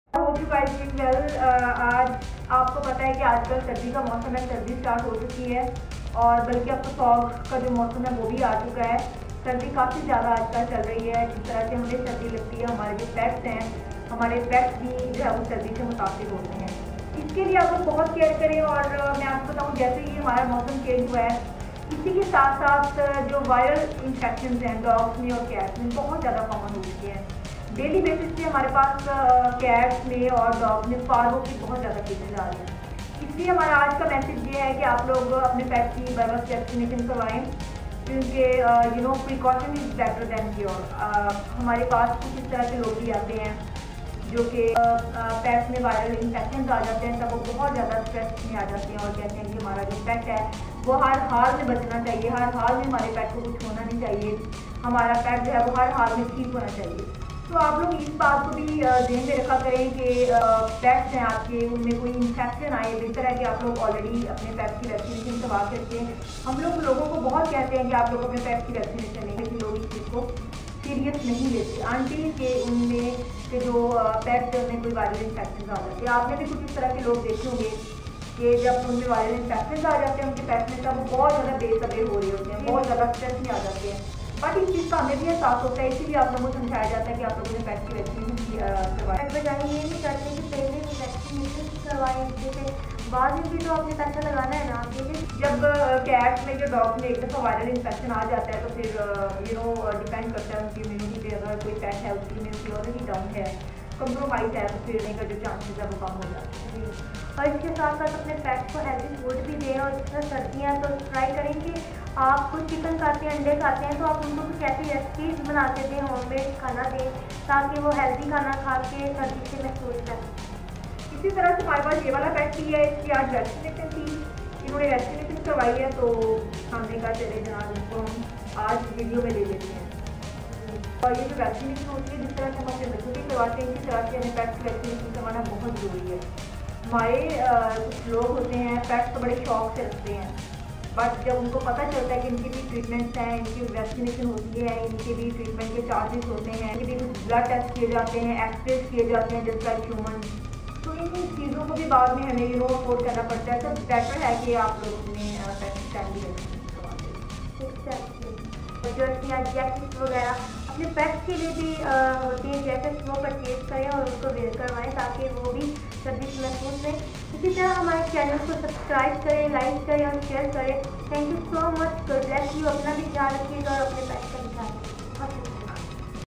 Pets & winter| سردی Pets اور وائرل انفیکشنز|Pets care|pets love
pets can be affected in winter, nowadays viral infections are very common in pets. if pets are vaccinate the chances of viral infection become less. Its better to vaccinate your pets.take care and love your pets.